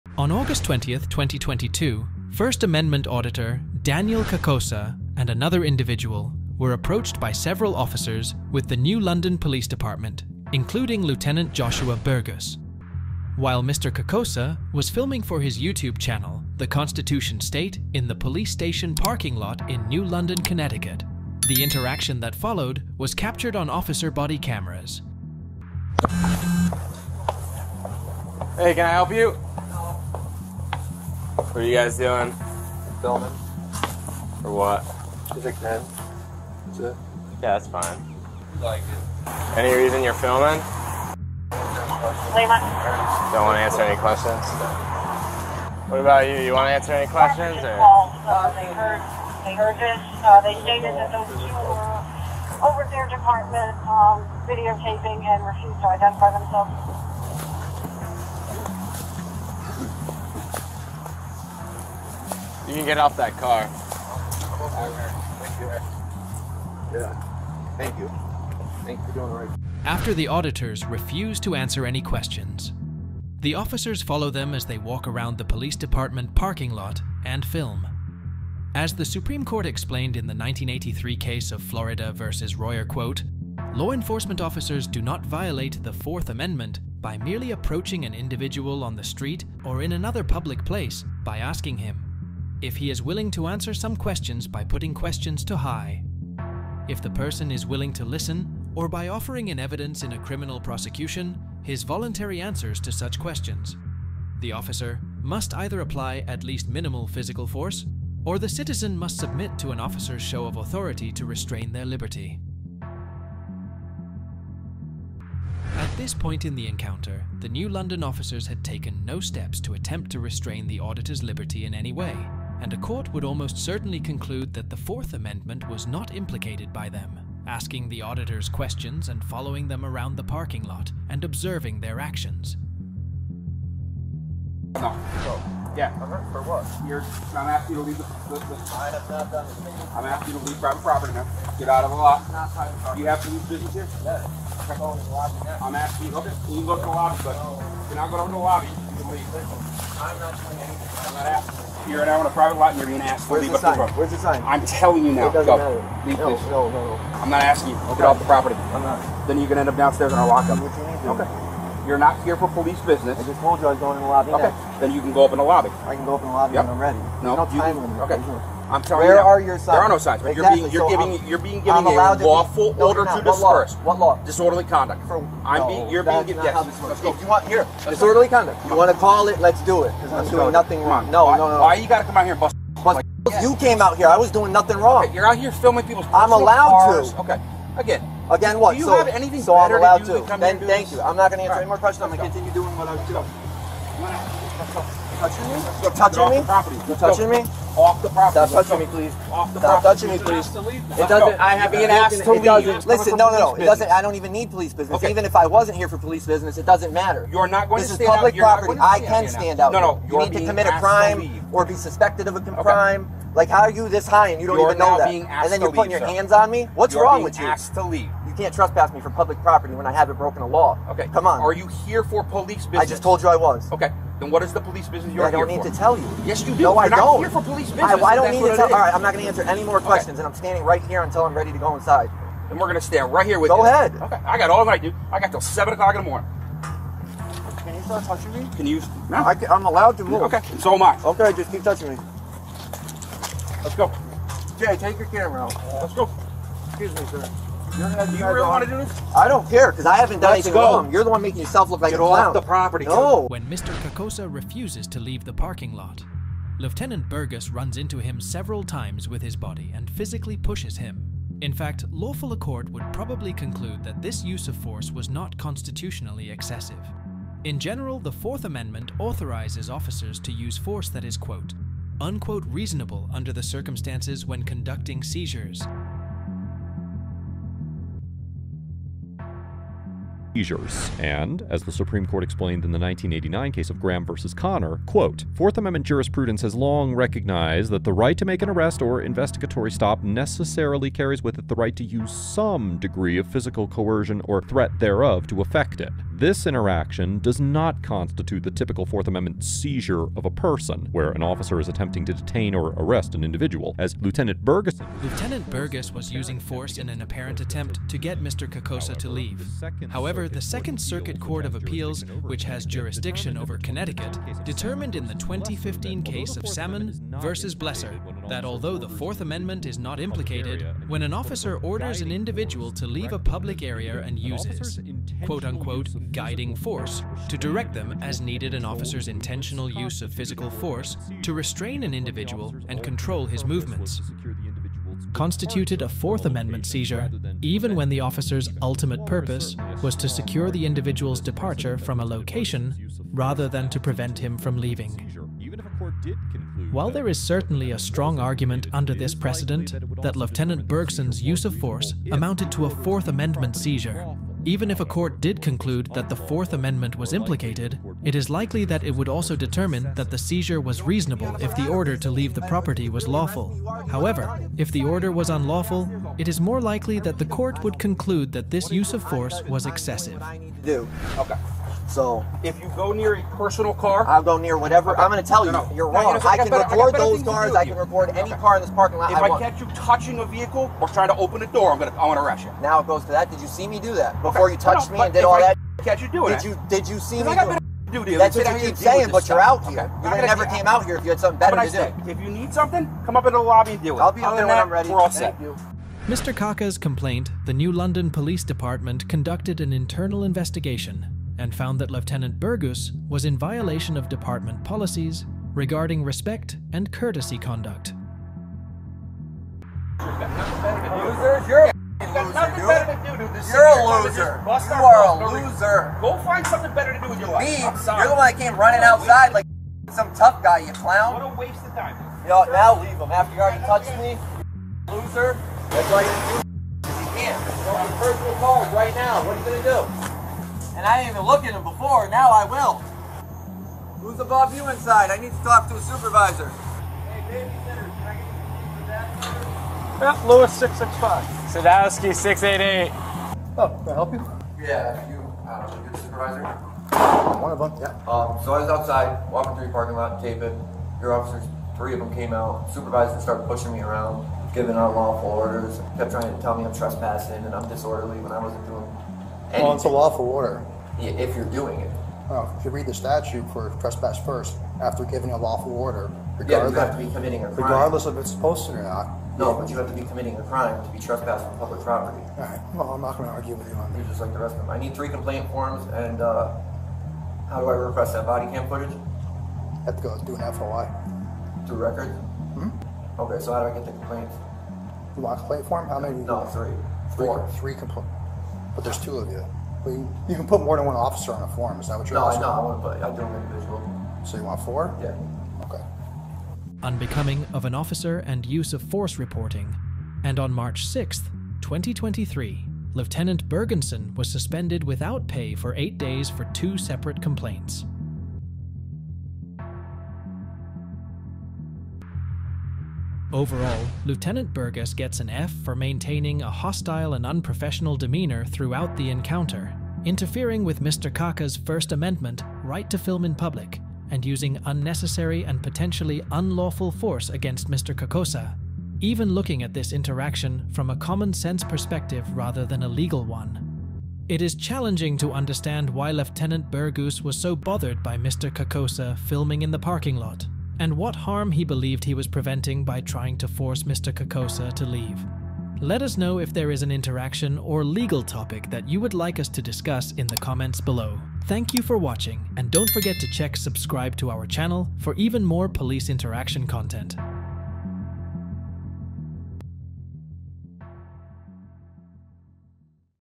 Corrupt Cop Is EXPOSED After This Trafic Stop
Corrupt Cop Goes To PRISON After This Stop

Prepare to be shocked as we uncover the truth behind corrupt cop Lieutenant Joshua Bergus in "Corrupt Cop Goes To PRISON After This Stop!!!" Join us as we delve into the disturbing encounter between First Amendment auditor Daniel Kakosa and officers from the New London Police Department, captured on body cameras.

On August 20th, 2022, Daniel Kakosa and another individual were approached by officers while filming for Kakosa's YouTube channel, Constitution State, in the police station parking lot. What follows is a shocking display of abuse of power as Lieutenant Bergus physically pushes Kakosa multiple times, violating his rights and using excessive force.

Explore the legal implications of this encounter as we dissect the Fourth Amendment and analyze the court precedents surrounding police seizures and the use of force. Despite an internal investigation by the New London Police Department and a suspension without pay for Lieutenant Bergus, justice prevails as we witness the consequences of his hostile and unprofessional conduct.

Witness firsthand the importance of upholding citizens' rights and holding corrupt cops accountable for their actions.